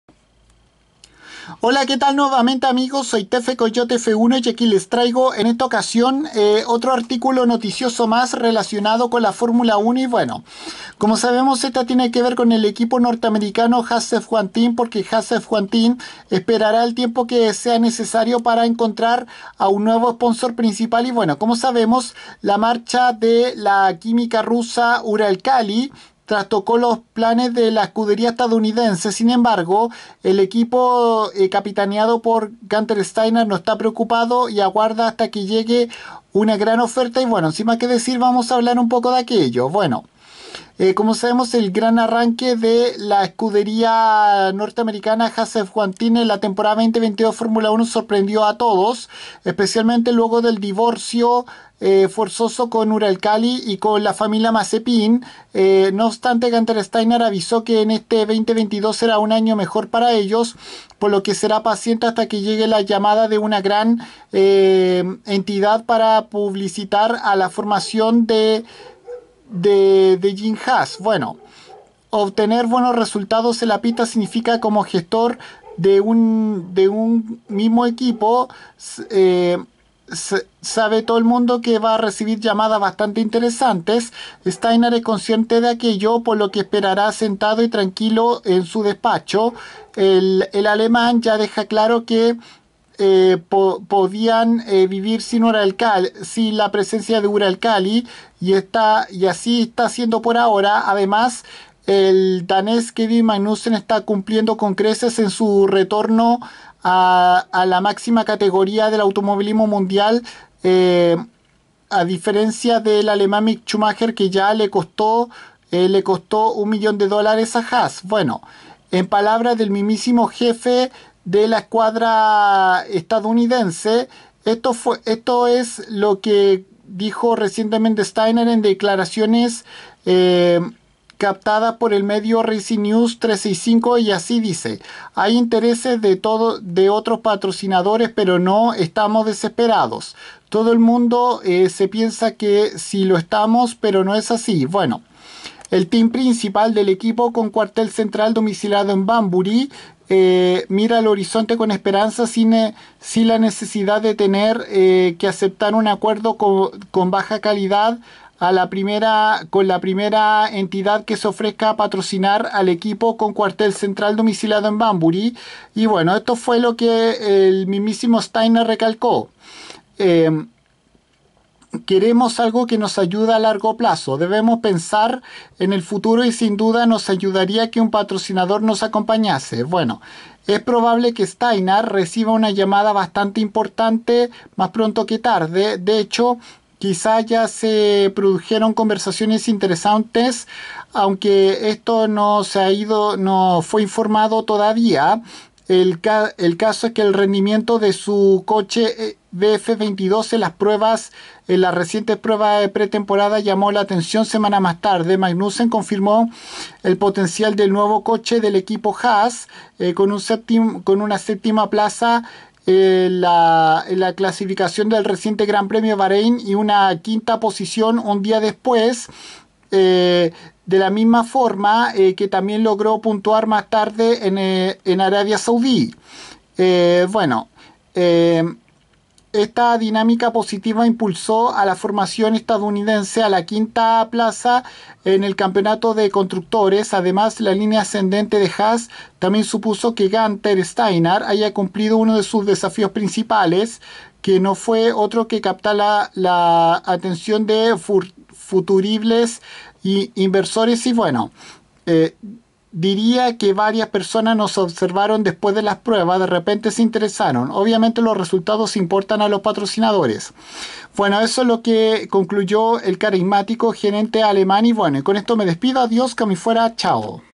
Haas F1 Team esperará el tiempo necesario para encontrar un nuevo sponsor principal
El equipo estadounidense luego de la desvinculación de la química rusa Uralkali y de la familia Mazepin dice a través de su máximo responsable Gunther Steiner que no están preocupados por aquello hasta que reciban una oferta